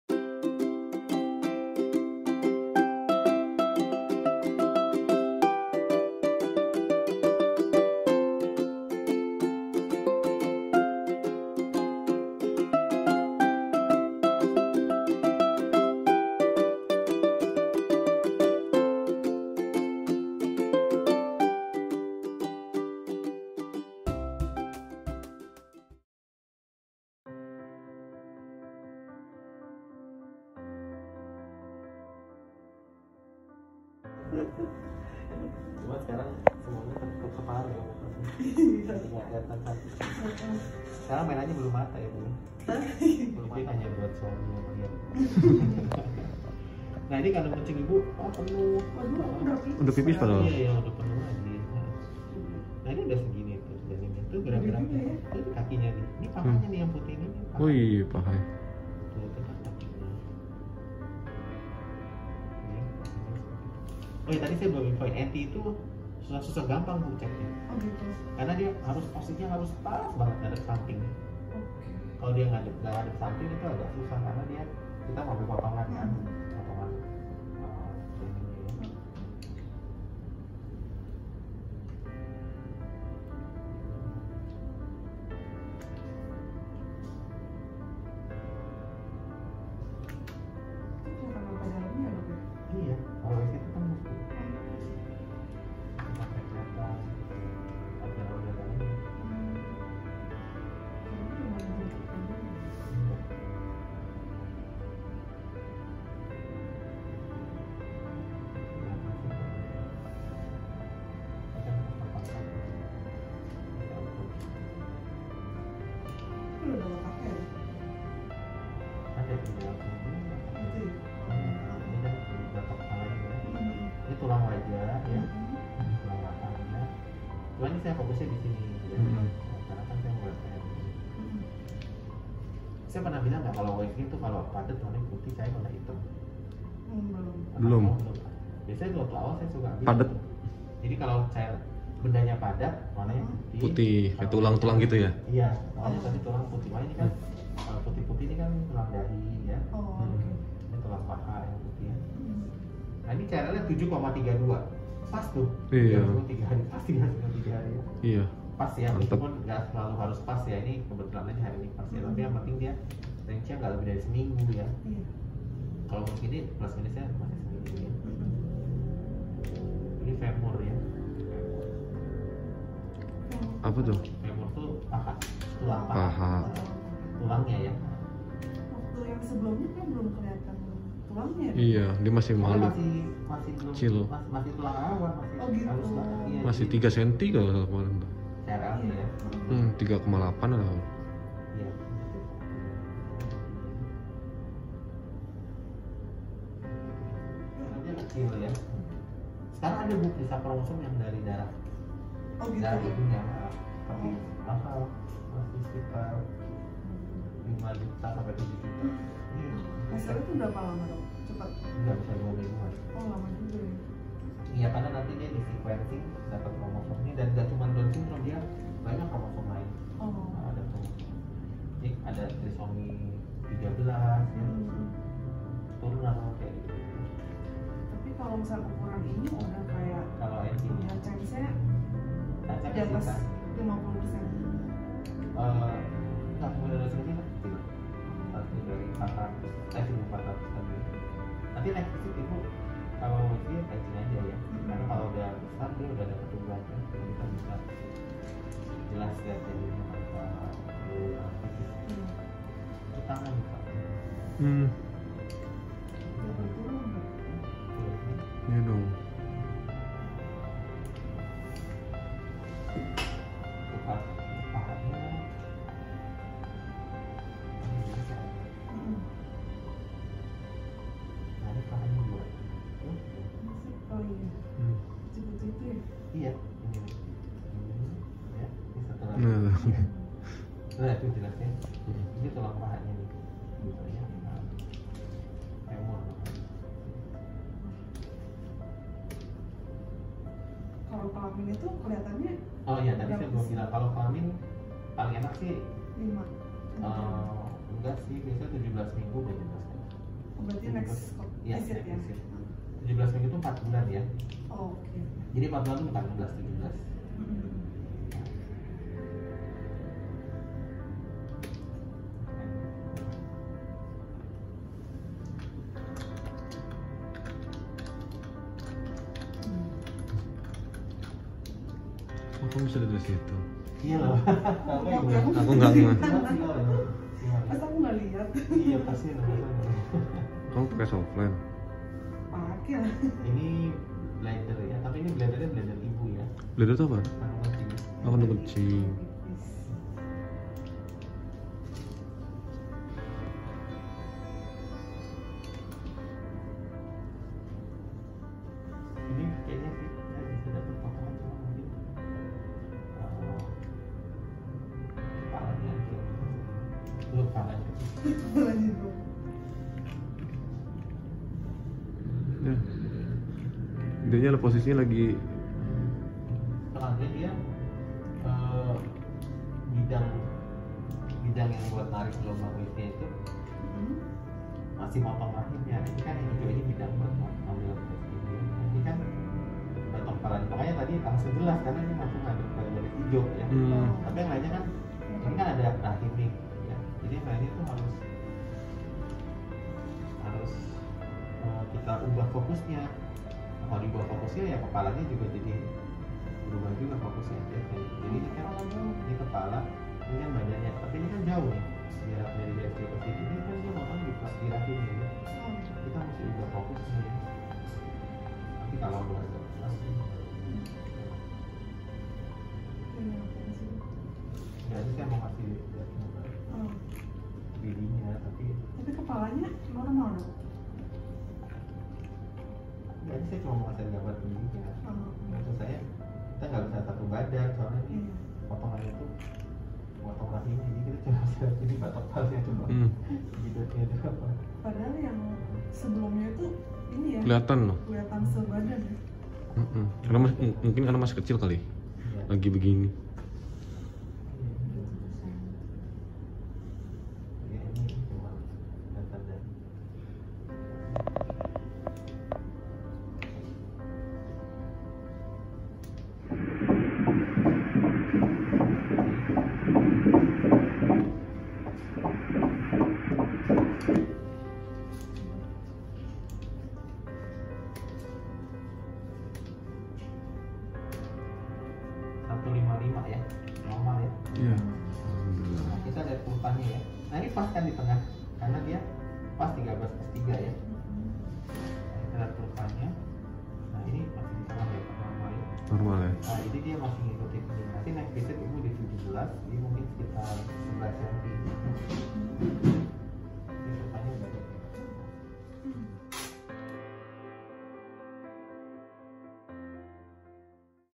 CEK USG KEHAMILAN 13 MINGGU | Zoey
Halo semua,

Aku mau share video dokumentasi pas waktu cek kandungan istriku yang udah 13 minggu, tanggal 23 Februari 2022 di RSPI Puri Indah - Jakarta.

Zoey sudah semakin keliatan bentuknya, keliatan kaki dan badan nya. Pas mau diliatin 3D nya belum bisa keliatan banget karena posisi nya Zoey gak bagus jadi gak bisa kena sama USG nya.

Alhamdulillah Zoey tumbuh dengan baik semua normal.

Thanks
Keluarga Thomas